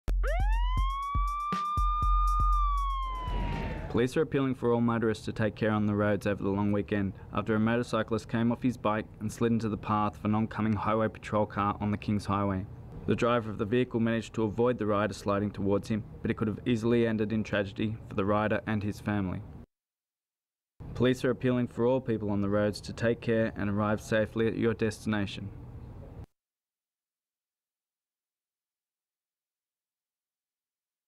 Motorcycle crash filmed by NSW Police highway patrol car
NSW Police have release confronting in-car vision of a near miss on the road.
Police hope the vision, which was captured by a camera mounted on a highway patrol car on the NSW south coast, will serve as a stark reminder to motorists about the dangers on our roads.
The in-car vision (ICV) shows an incident on the Kings Highway near Batemans Bay involving an 18-year-old learner motorcyclist yesterday morning.
The motorcyclist lost control in wet conditions, fell to the road and slid into the path of the on-coming highway patrol car.
The highway patrol officer had only a split-second to react but managed to avoid hitting him.
The rider was shaken but otherwise unhurt.
Operation Commander for Traffic Services, Acting Detective Superintendent Bernie Ryan said the incident shows how quickly and easily things can go wrong on the road.
"This has been a massive wake-up call for the rider involved and we think it should also be one for every other driver on the road.
"It is miraculous the highway patrol officer managed to avoid hitting the rider. His skill in avoiding an almost certain fatal collision is worthy of praise.
"I have no doubt that had this rider slid in front of a driver breaking the speed limit he would have been seriously injured or killed," Det Supt Ryan said.
"This incident occurred in adverse weather conditions which are similar to those expected across most of the state over the long weekend.
"I cannot stress enough the importance of slowing down in the wet and driving to suit the conditions.
"The inevitable reality is other drivers are not going to have the same luck as this rider and be given a second chance -- don't let that be you!